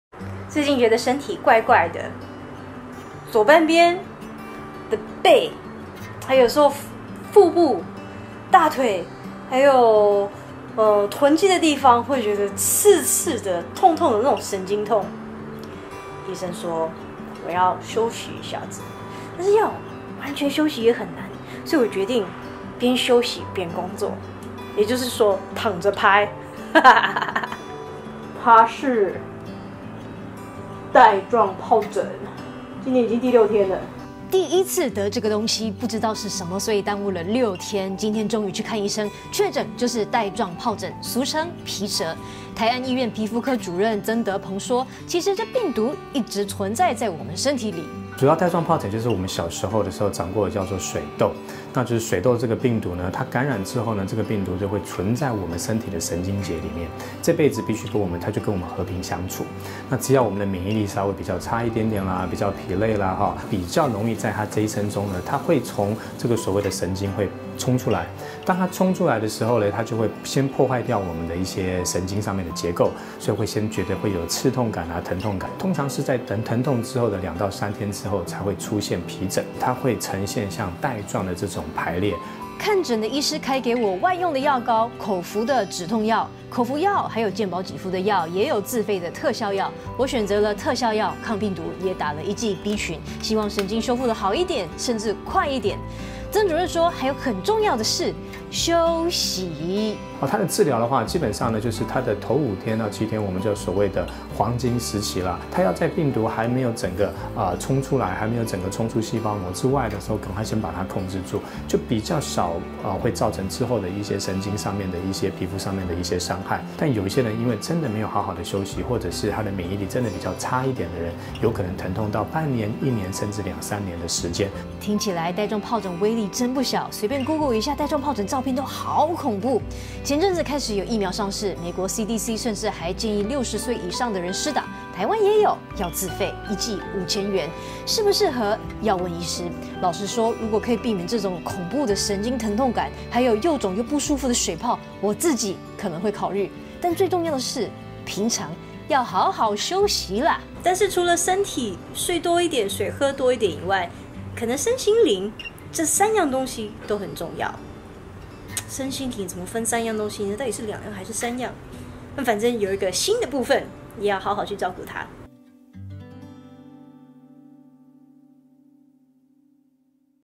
20180703 帶狀皰疹 醫師: 黃金5-7天趕緊治療
我也中了

接下來得研究除了吃和睡 如何提升免疫力...